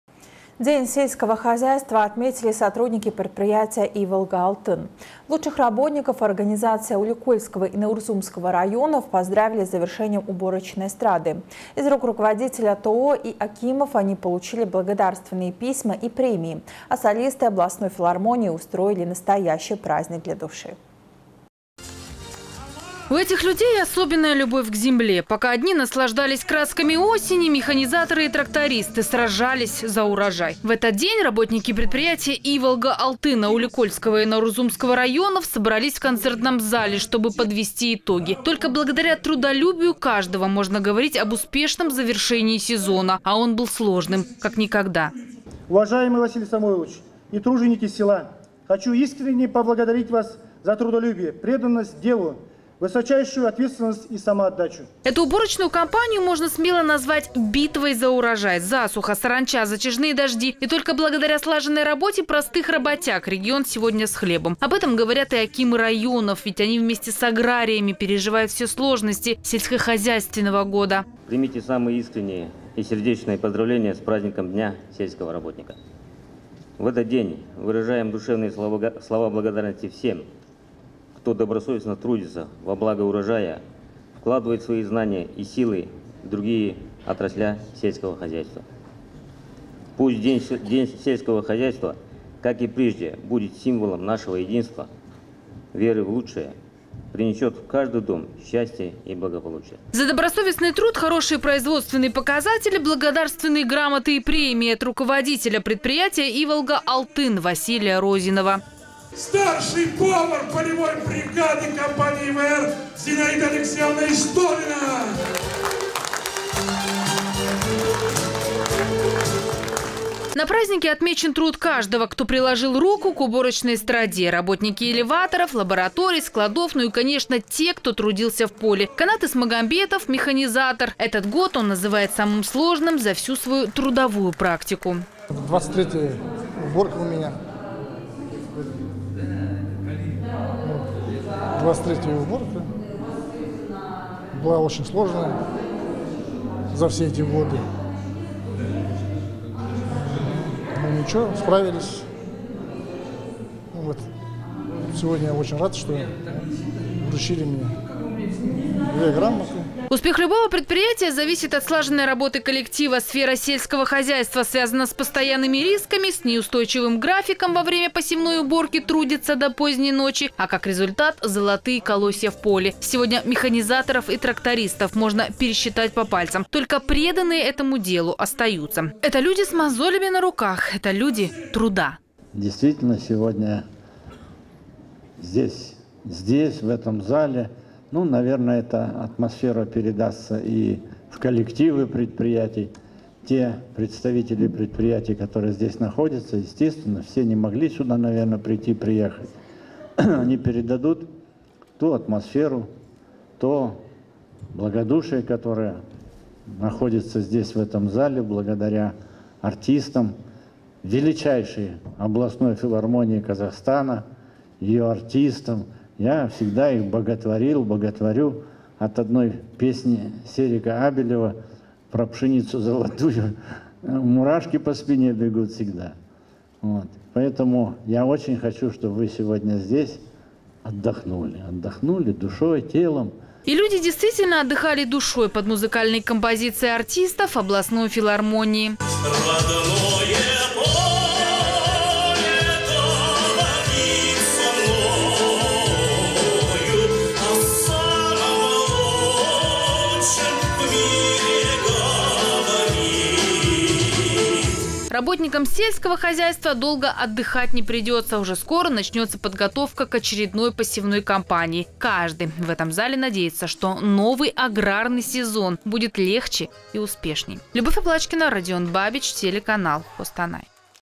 Праздник работников сельского хозяйства
День сельского хозяйства отметили сотрудники  предприятия «Иволга — Алтын». Лучших работников организаций Аулиеколького и Наурзумского районов поздравили с завершением уборочной страды. Из рук руководителя ТОО и акимов они получили благодарственные письма и премии, а солисты областной филармонии устроили настоящий праздник для души.